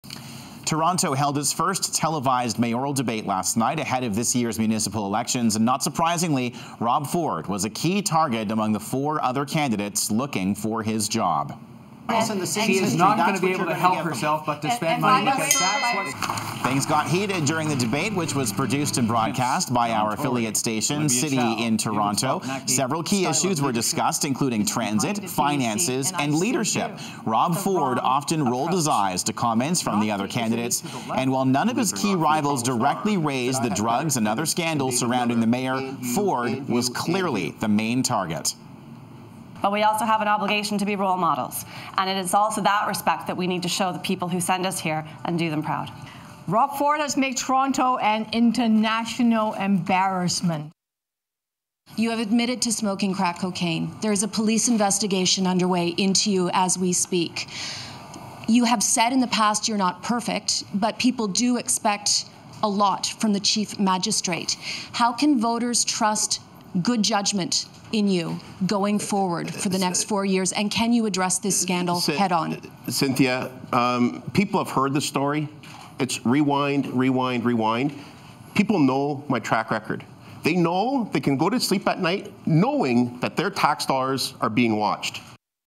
BT Vancouver: Toronto Mayoral Race First Televised Debate
Toronto held its first televised mayoral debate last night. Not surprisingly, Rob Ford was the target among the four other candidates looking for his job. None of his rivals directly raised the topic of scandals, but Ford was clearly the main target.